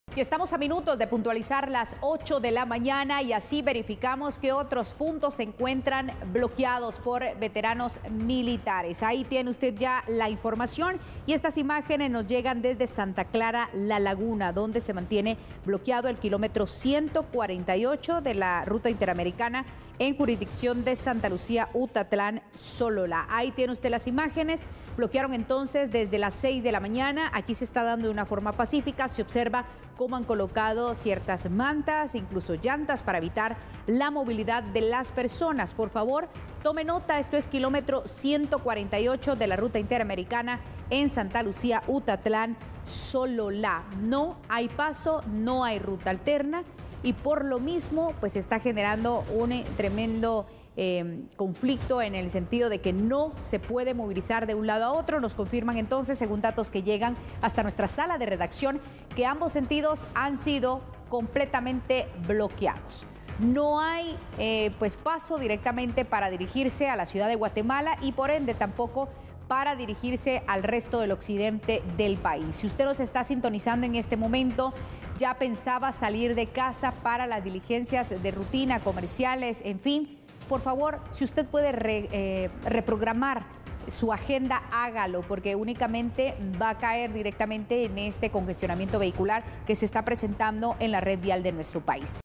Bloquean km.148 de la Ruta Interamericana en Sololá
Esta mañana, veteranos militares del Municipio de Santa Clara La Laguna bloquearon el kilómetro 148 de la Ruta Interamericana en jurisdicción de Santa Lucía Utatlán, Departamento de Sololá.